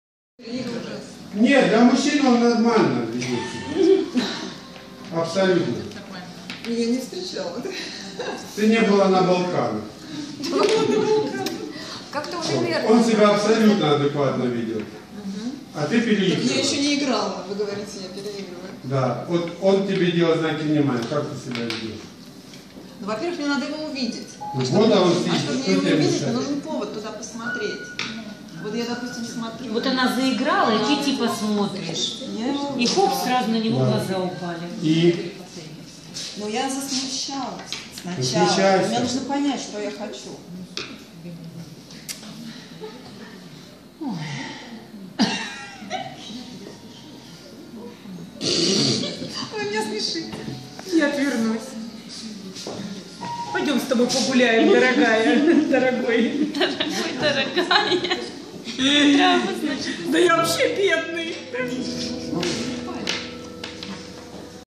"Как мы снимали фильм в Alta Galerie Vín" -3 серия.
Подготовка к процессу, папарацци в акции, все в напряжении ждут, чем это все закончится